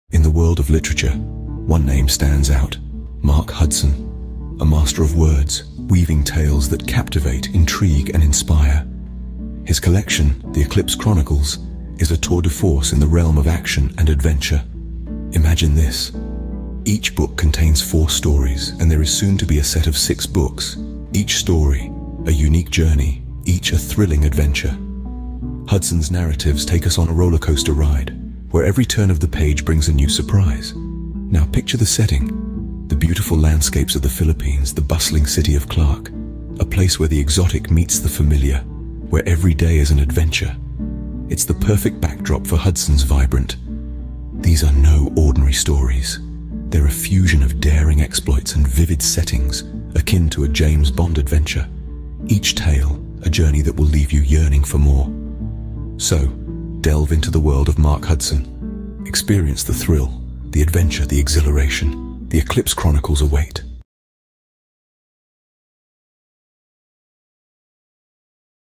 Mark Hudson, The Eclipse Chronicles.
The Eclipse Chronicles is a new book from a new author Mark Hudson.
Each book has a set of four gripping adventures set in paridise.
There will be six books in the set to start with a few for a couple more to be added later on.
The stories read like a high action adventure that would make a great TV show.